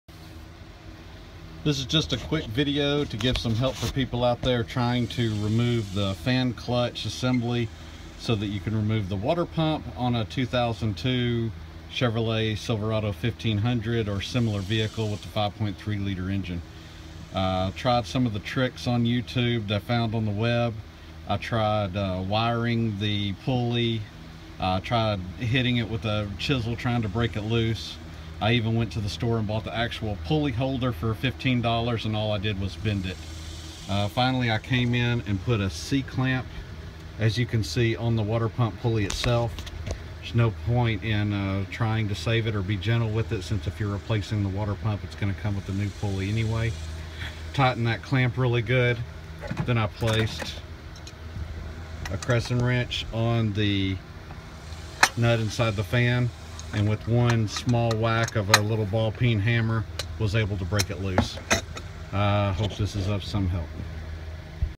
Removing fan clutch 2002 Silverado 5.3
Water pump or fan clutch removal LS 5.3 2002 Silverado 1500.
My way of removing the very stubborn fan clutch assembly from the water pump on a 5.3 liter LS engine. This is a 2002 Chevy Silverado 1500.